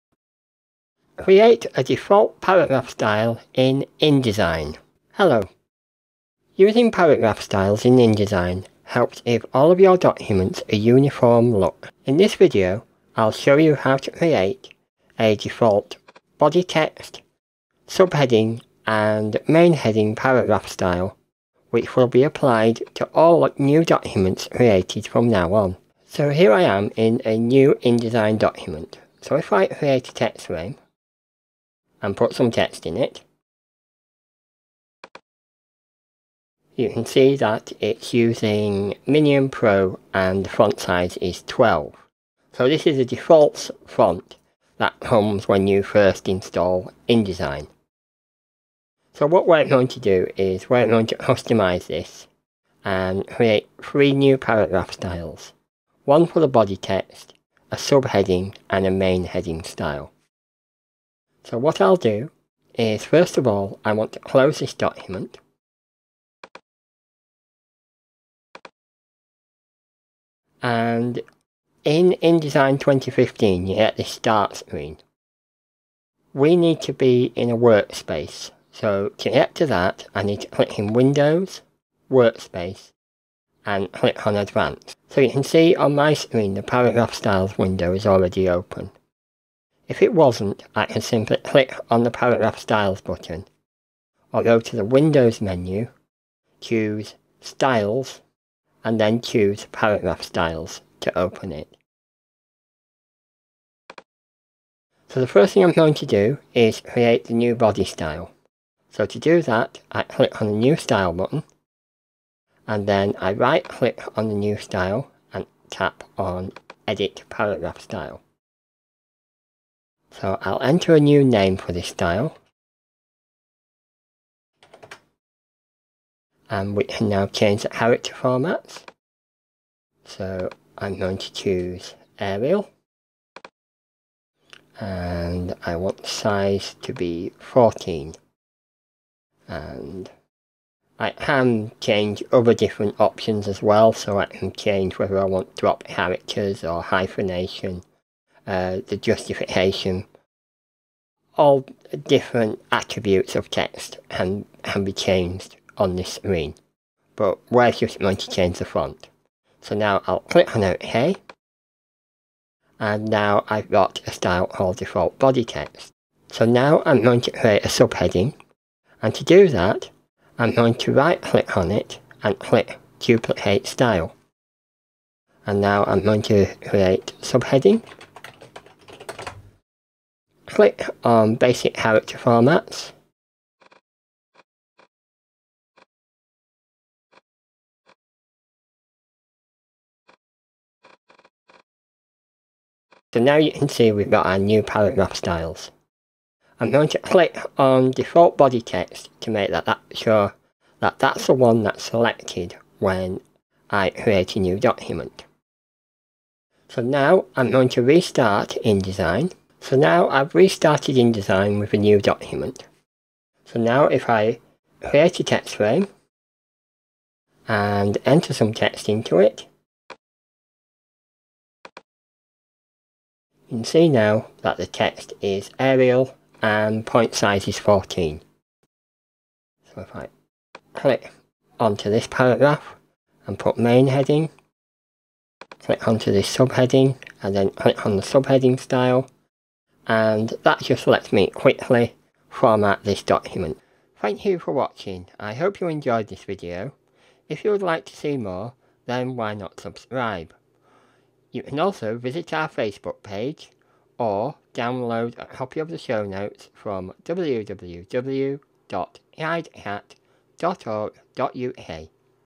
InDesign - Setting the default font.
Using paragraph styles in InDesign helps give all your documents a uniform look and feel.  In this video I will show you how to setup default styles for Body Text, Subheadings and Main Heading which will be applied to all new documents.